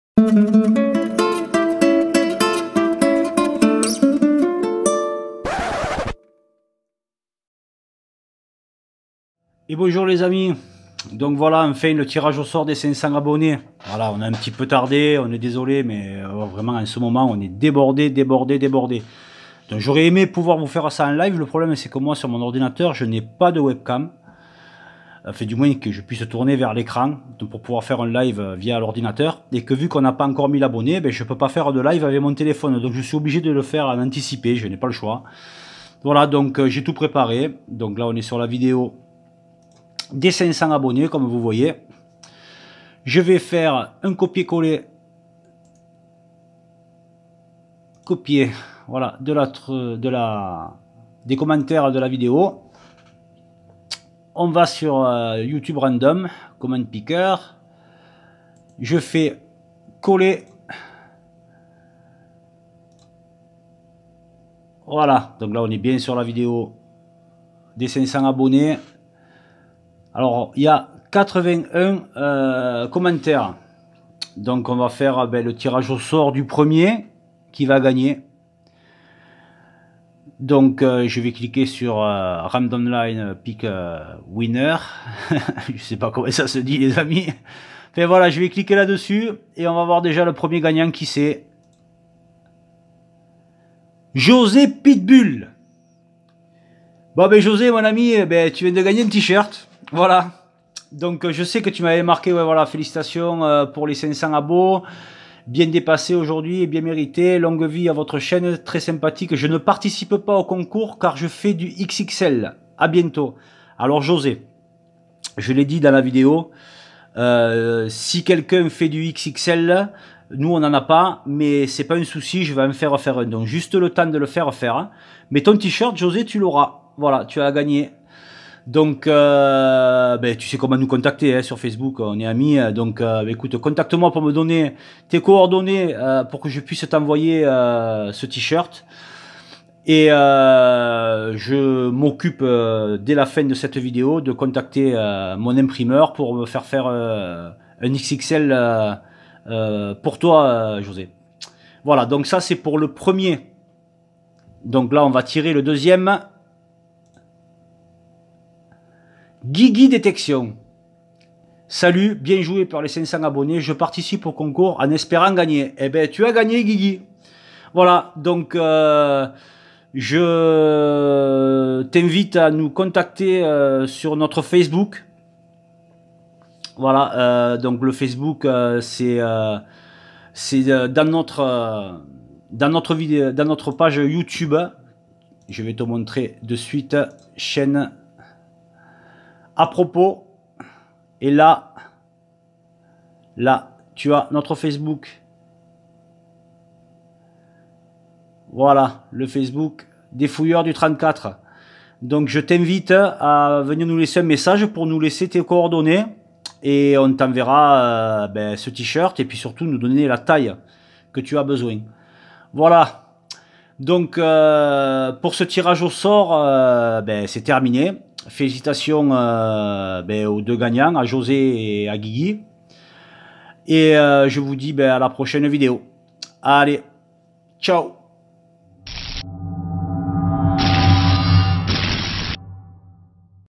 tirage au sort du jeu concours 500 abonnés
salut les amis félicitations au deux gagnant

voici le lien de notre page facebook pour me laisser vos coordonnées et la taille que vous faite :-)

ou sur notre compte facebook stef steff

a bientot les amis et bonne détection ;-)